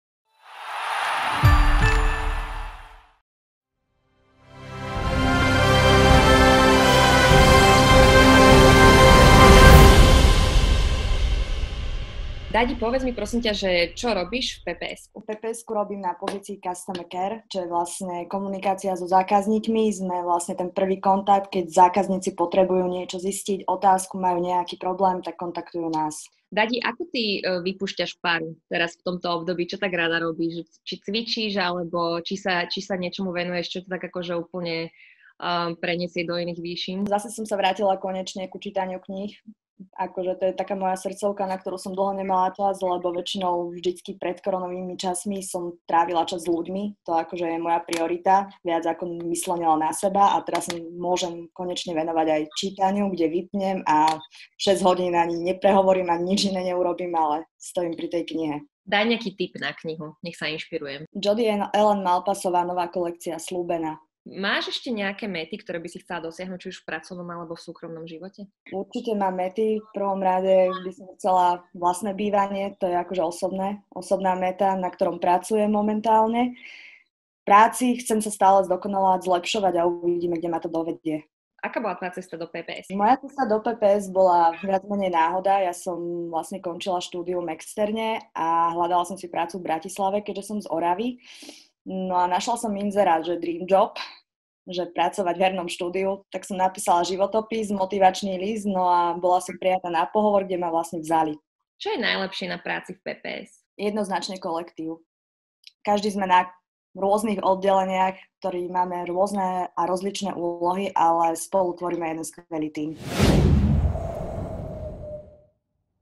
PPS People: Dada "DADUSH" Dohnalová
PPS People is here folks! We have grown fond of this mini series and we hope you too! Dada, our customer care princess is here with her calm presence, after all dealing with customers she kinda has to be, although she is not thinking about joining the Himalayan monks. yet! Do you wish to know what she does to let go of steam? And what book is she reading? You will be surprised!